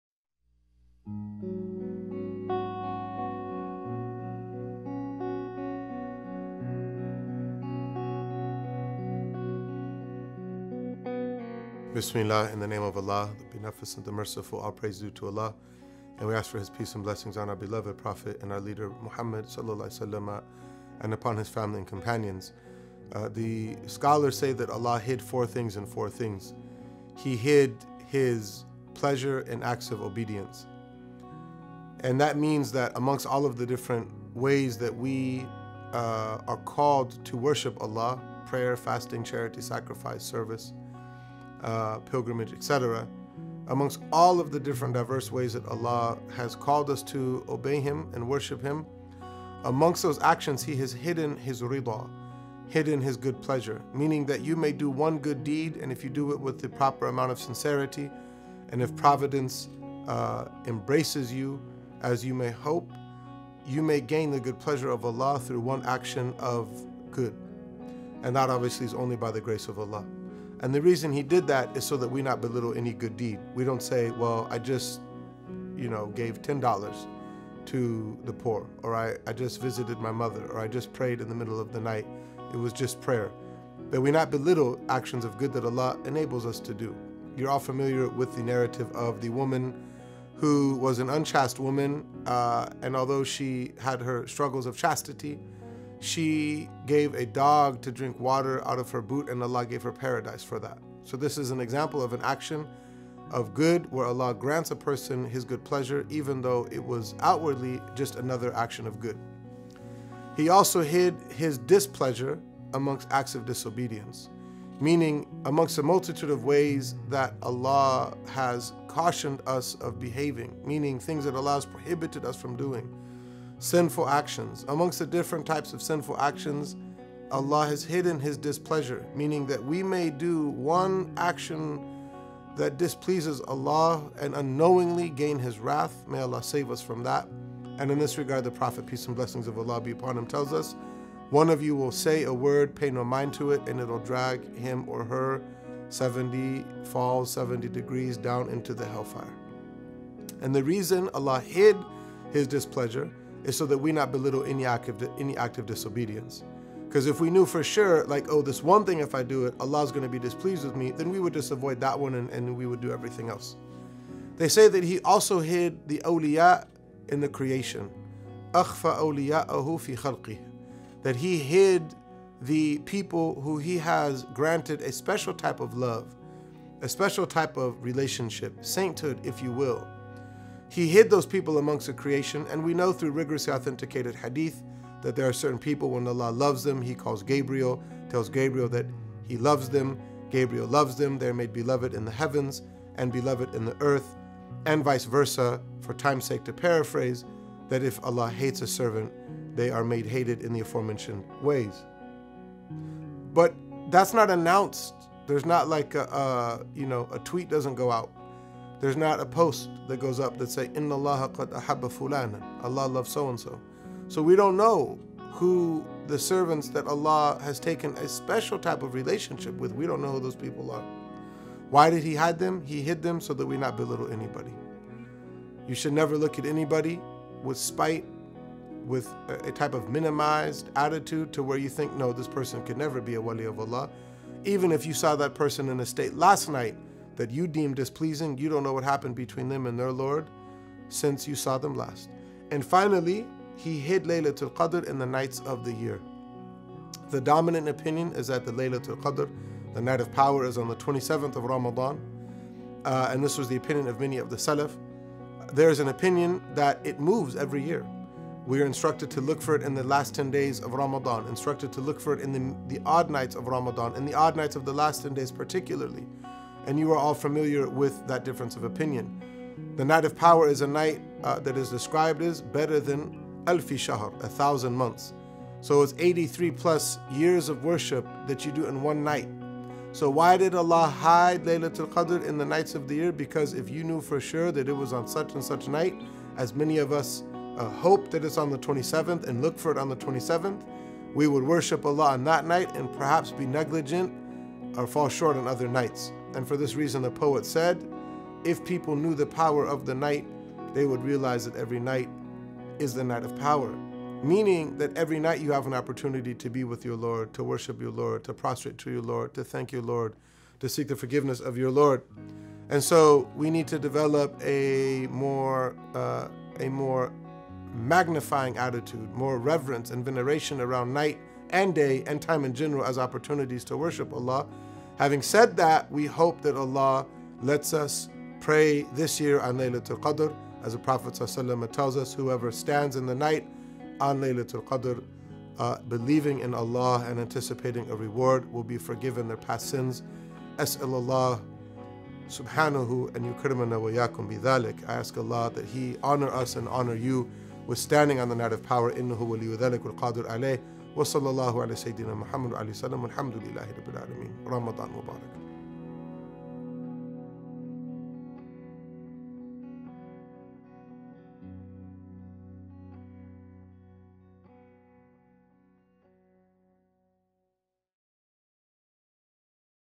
Ramadan Reminders with Usama Canon :: (16) Four in Four
RAMADAN REMINDERS (16) "Four In Four"

Day 16 of the Ta'leef “Ramadan Reminders” Usama Canon relates the saying of the scholars of Islam that Allah hid four things in four things.

Help us reach our fundraising goal this month by joining the Ta’leef Ramadan Campaign.